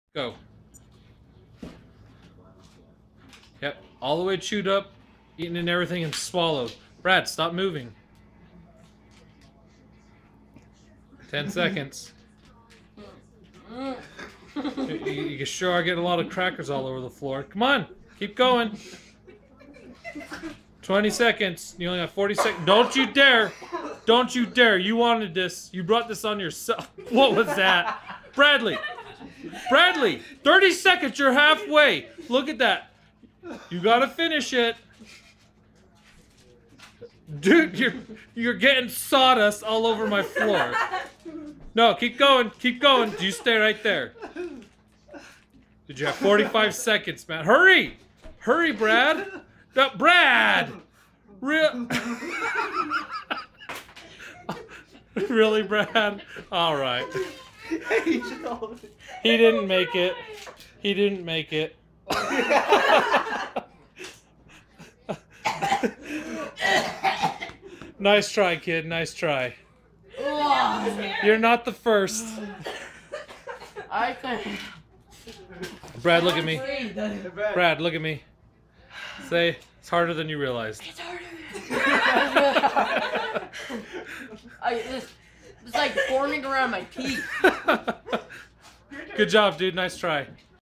Brad tries the MRE cracker challenge
Brad tries to eat a MRE cracker in under 1 minute.  He comes close but doesn't get it.